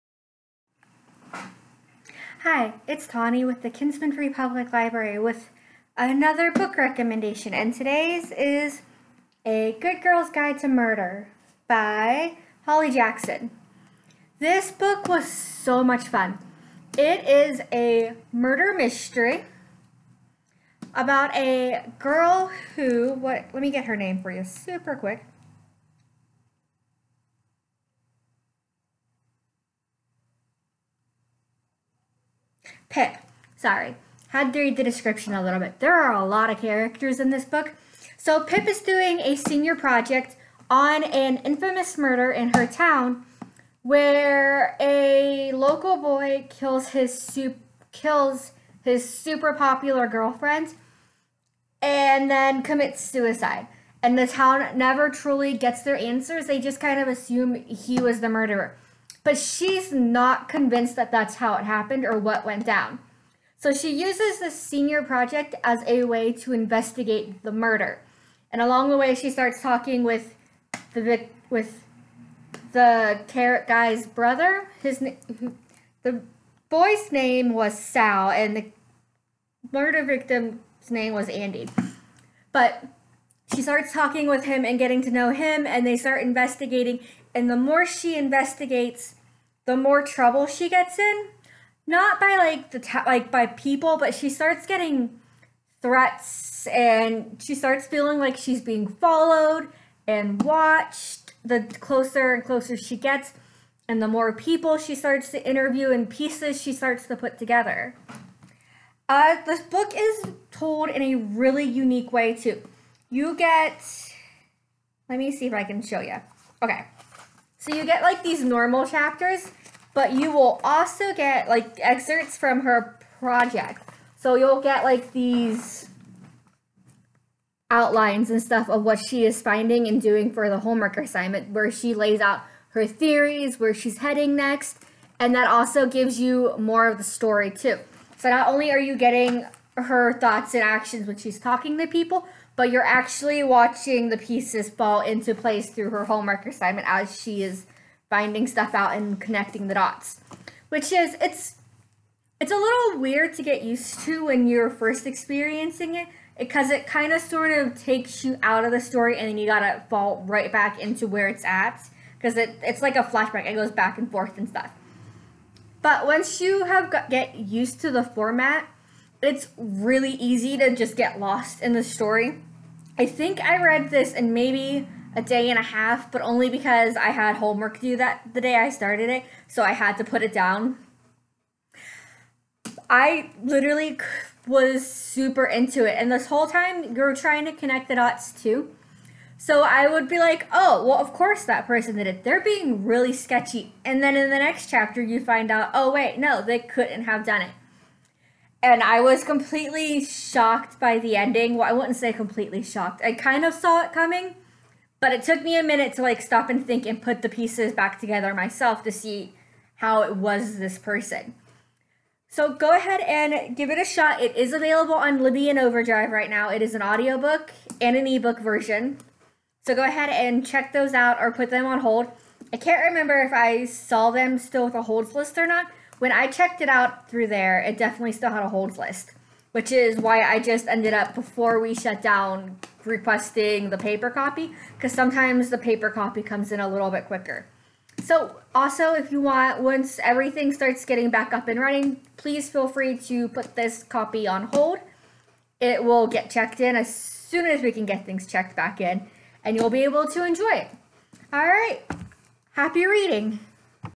Kinsman Free Public Library Talks Books: A Good Girl's Guide to Murder
A good murder mystery is exactly what is needed to distract. Holly Jackson comes through with her book A Good Girl's Guide to Murder. The story is fast paced well thought out and super easy  to fall into!

Check it out today through Libby or Overdrive.